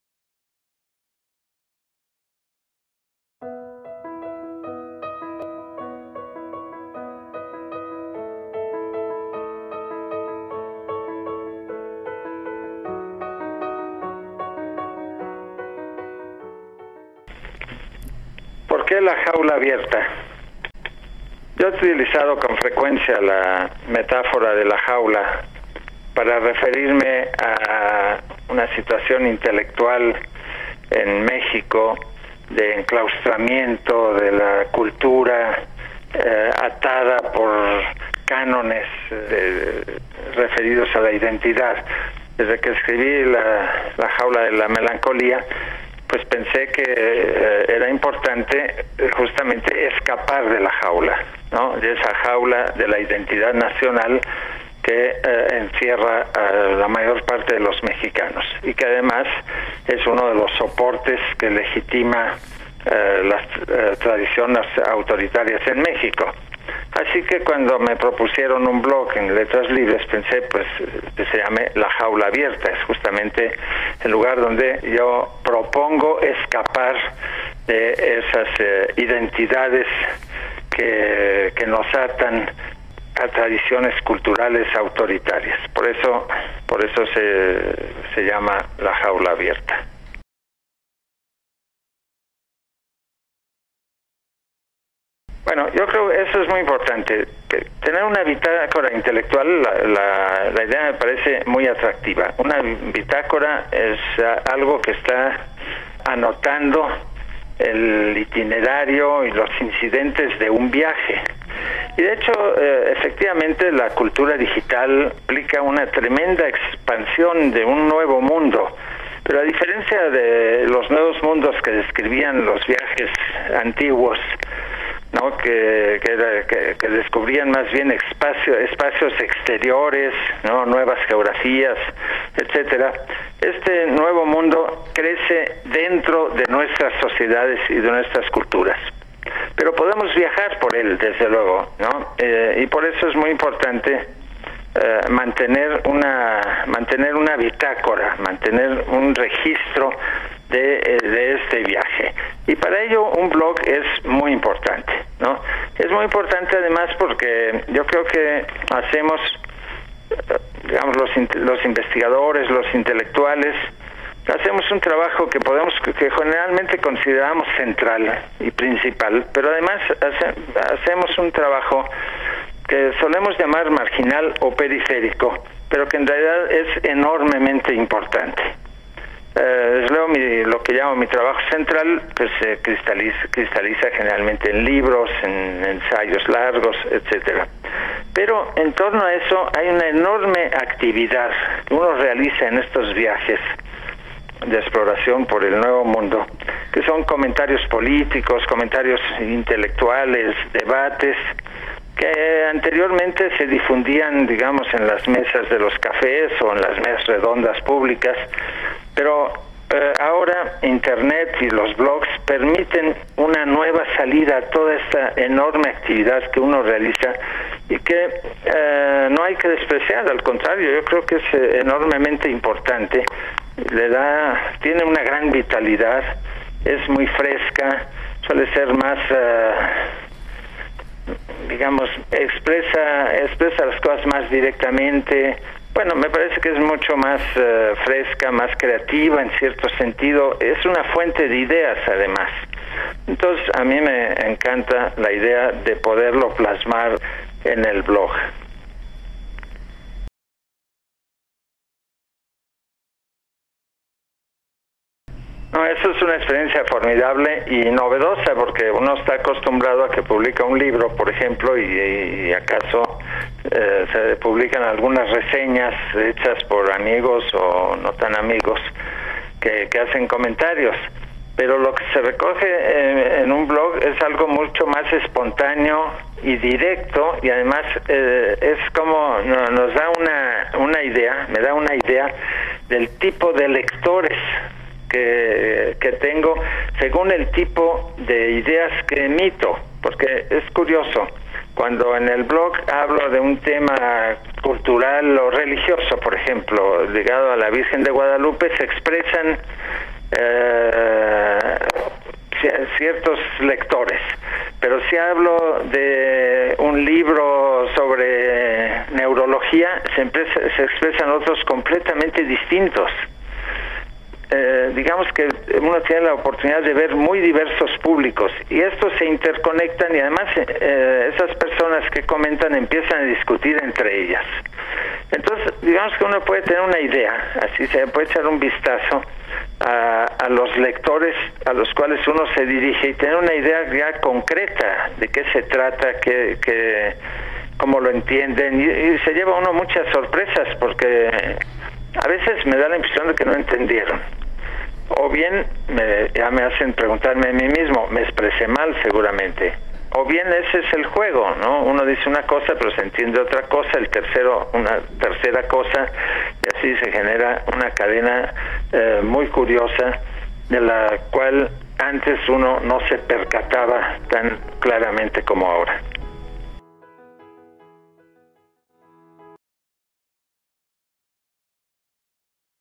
Roger Bartra habla de La jaula abierta, su blog en Letras Libres
Roger Bartra es doctor en sociología por La Sorbona y se formó en México como etnólogo en la Escuela Nacional de Antropología e Historia.

Escribe La jaula abierta, en letraslibres.com, desde el 29 de agosto de 2007.